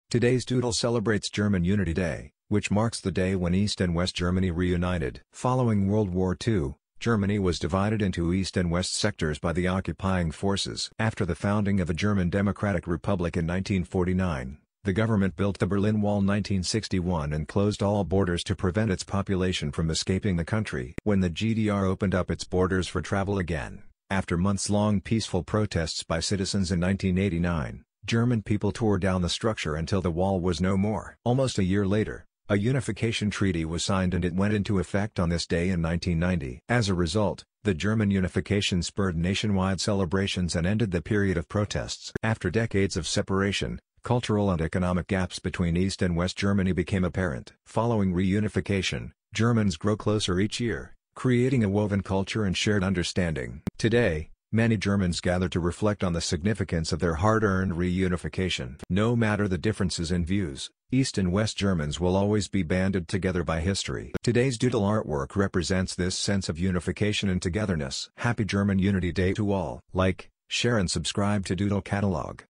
Tag der Deutschen Einheit | 32. Tag der Deutschen Einheit
Google Doodle :
32. Tag der Deutschen Einheit

Tag der Deutschen Einheit

German Unity Day 2022

32. Tag der Deutschen Einheit

Today’s Doodle celebrates German Unity Day, which marks the day when East and West Germany reunited Following WWII, Germany was divided into East and West sectors by the occupying forces  After the founding of the German Democratic Republic in 1949, the government built the Berlin wall 1961 and closed all borders to prevent its population from escaping the country  When the GDR opened up its borders for travel again—after months long peaceful protests by citizens in 1989—German people tore down the structure until the wall was no more  Almost a year later, a unification treaty was signed and it went into effect on this day in 1990  As a result, The German Unification spurred nationwide celebrations and ended the period of protests After decades of separation, cultural and economic gaps between East and West Germany became apparent  Following reunification, Germans grow closer each year, creating a woven culture and shared understanding  Today, many Germans gather to reflect on the significance of their hard-earned reunification  No matter the differences in views, East and West Germans will always be banded together by history  Today’s Doodle artwork represents this sense of unification and togetherness Happy German Unity Day to all!